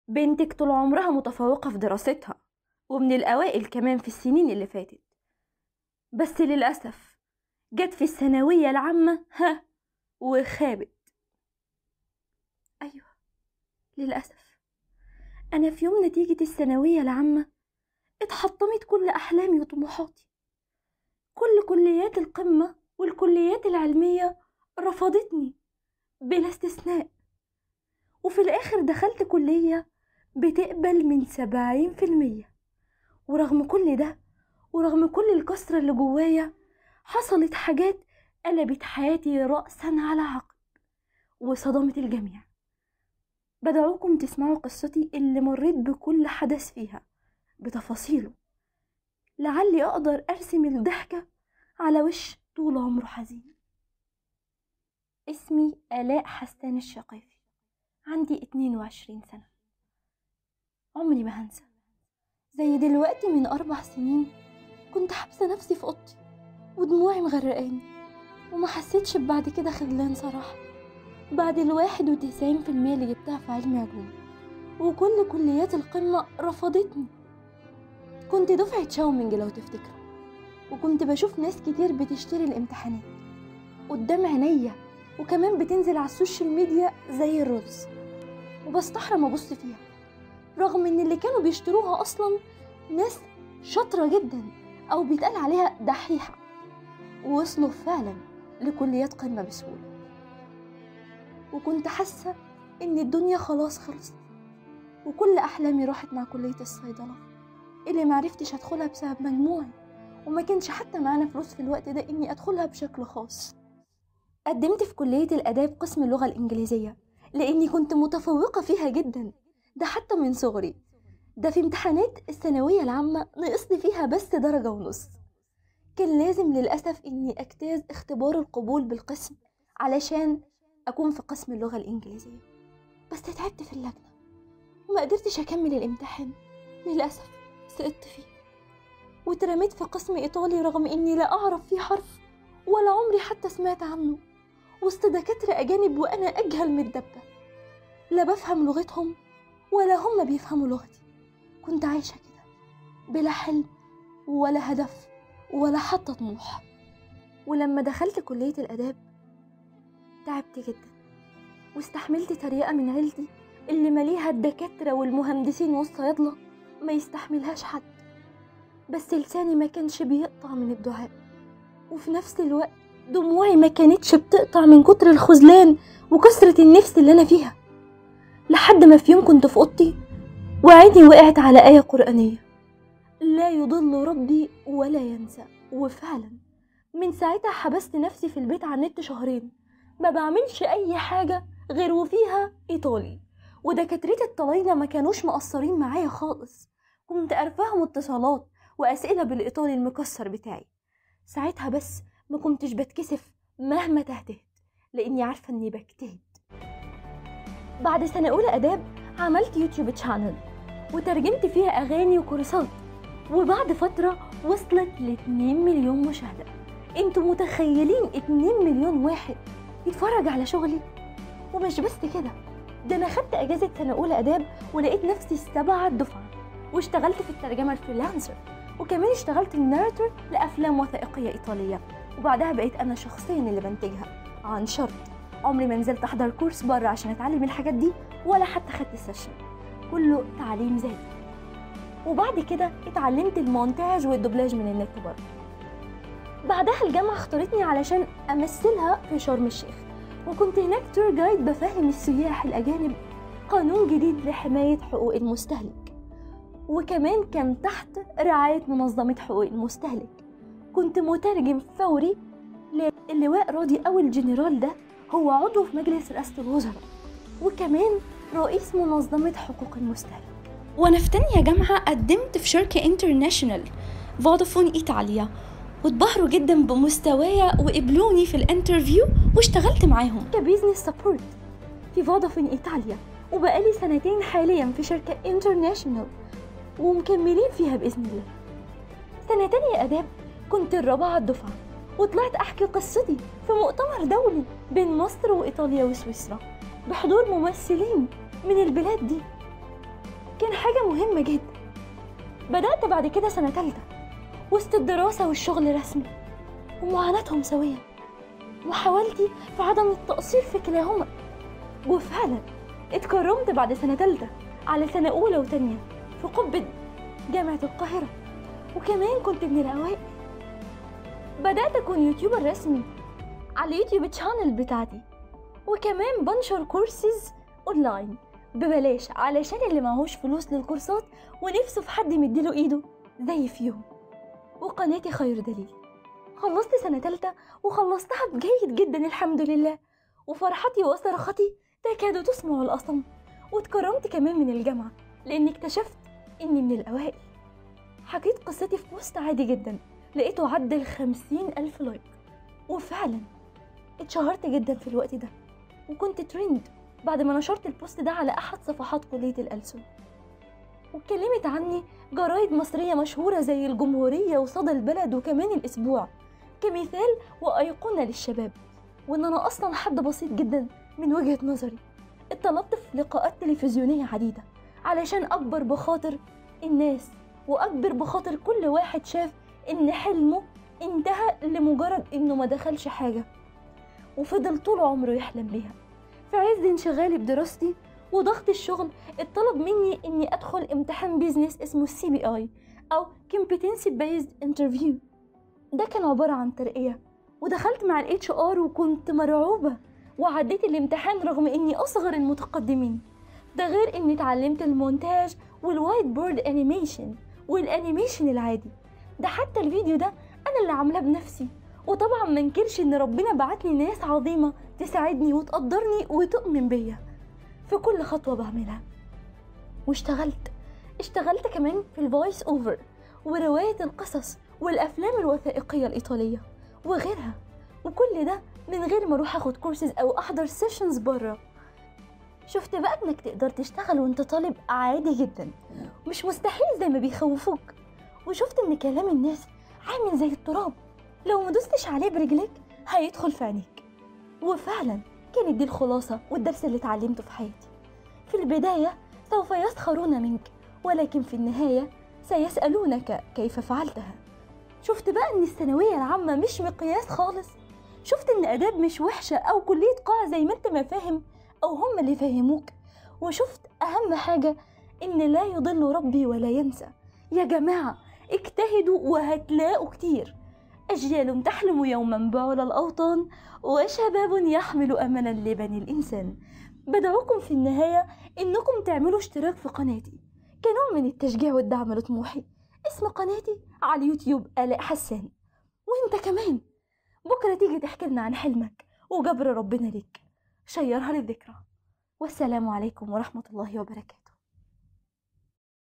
جبت مجموع وحش في ثانوية عامة ودخلت كلية مبحبهاش ولكن في النهاية صدمت الجميع!! آلاء حسان- قصة حياتي!
physics chapter 1 ثانوية عامة,
unit 1 ثانوية عامة,
ثانوية عامة 2019,
ثانوية عامة 2020 اليوم,
99 ثانوية عامة